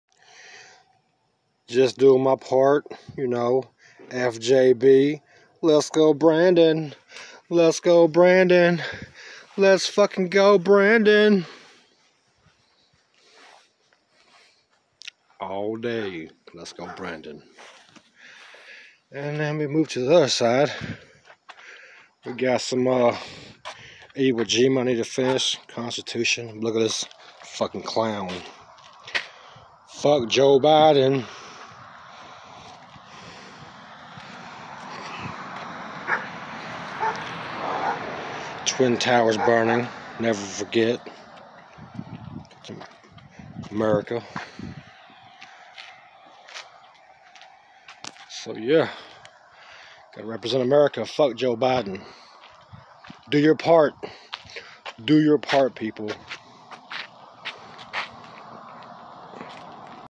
FJB-LET'S GO BRANDON - JUST DOING MY PART SPREADING THE LOVE.
my custom painted dodge ram sometimes you have to go above and beyond the norms to get the message across